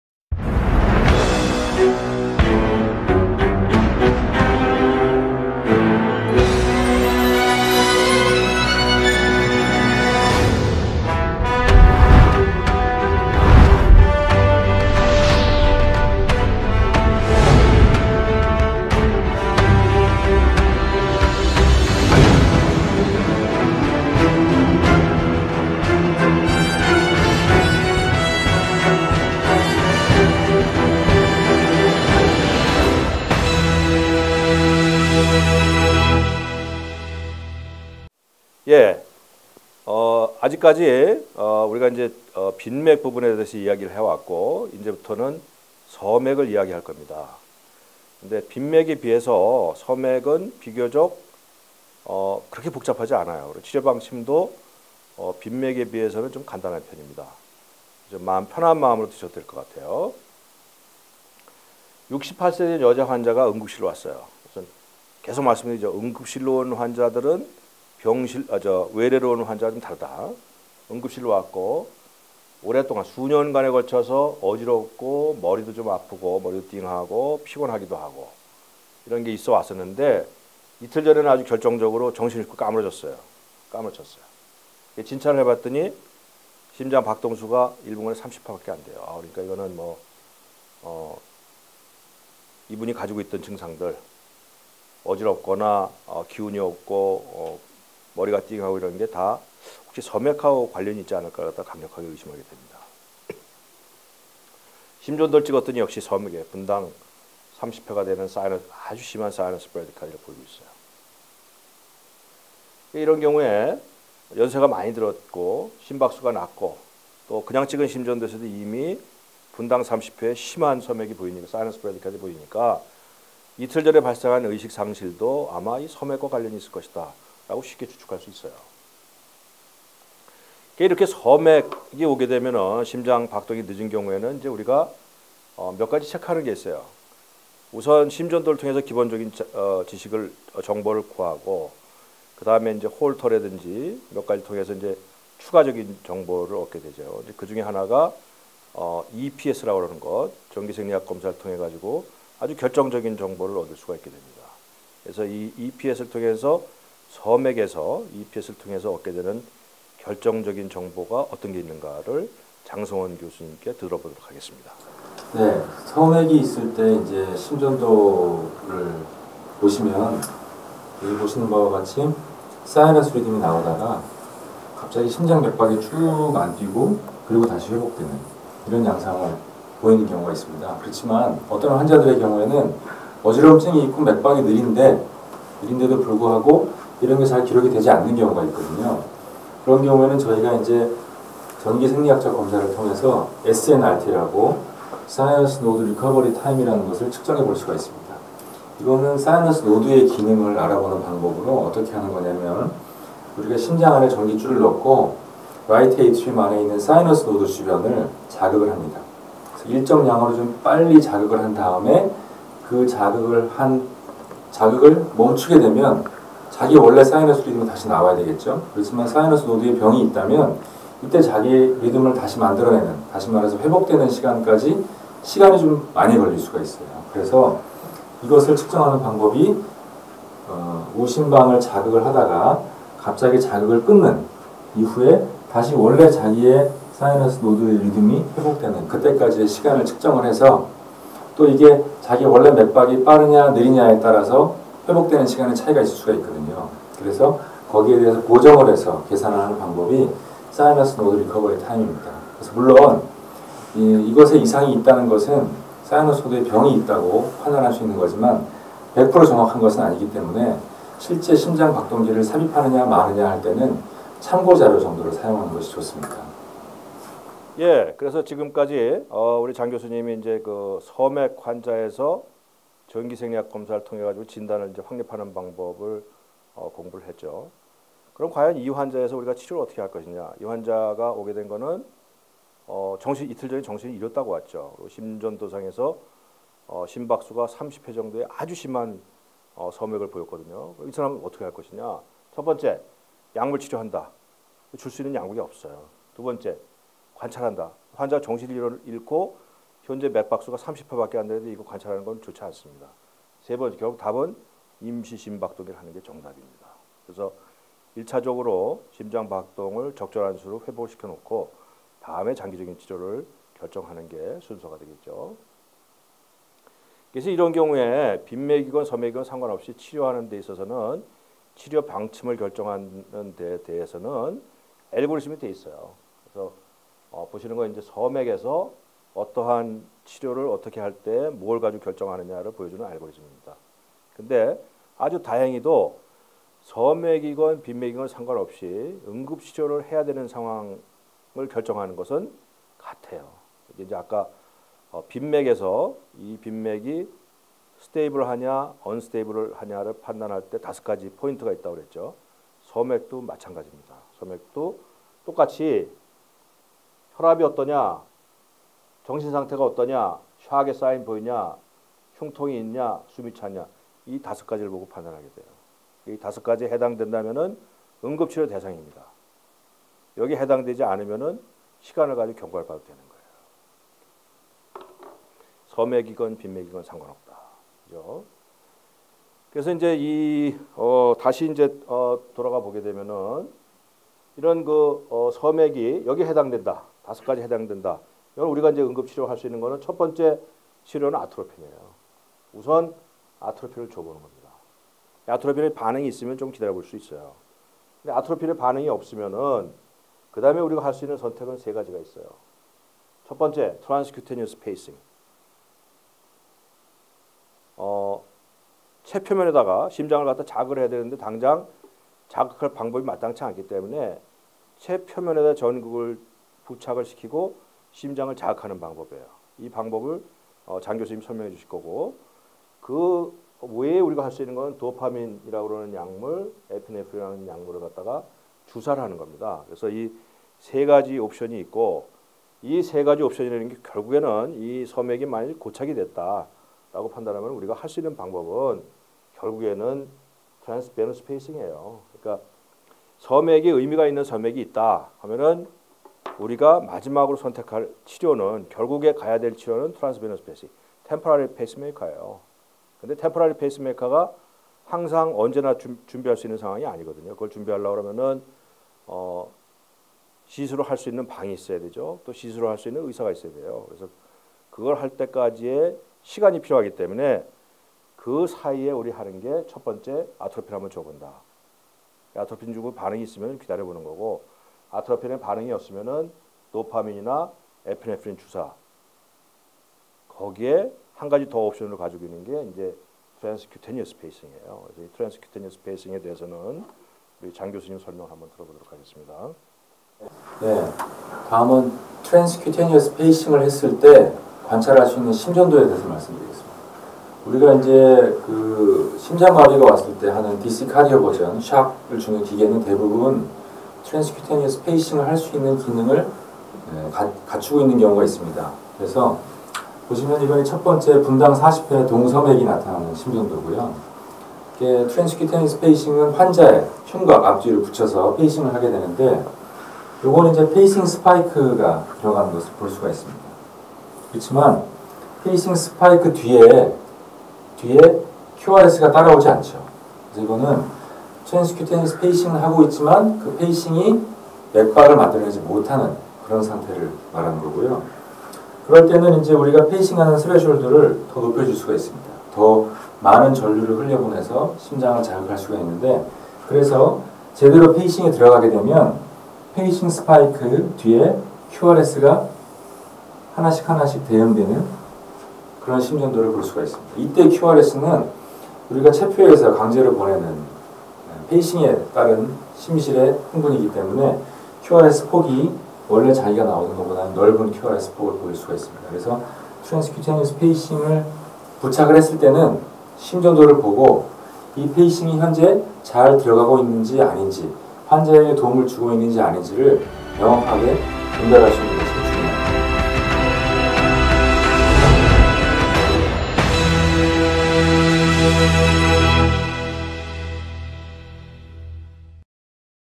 '부정맥에의 접근' (Approaches to pts with arrhythmias) #8 서맥부정맥 Sinus Node Dysfunction
English subtitle available. 노태호의 '알기 쉬운 심전도' 시즌 2 '부정맥에의 접근' 8편입니다. 이번에는 서맥에 대해 알아봅니다. 서맥의 주요 원인이 동기능부전증후군 sinus node dysfunction을 우선 공부합니다 .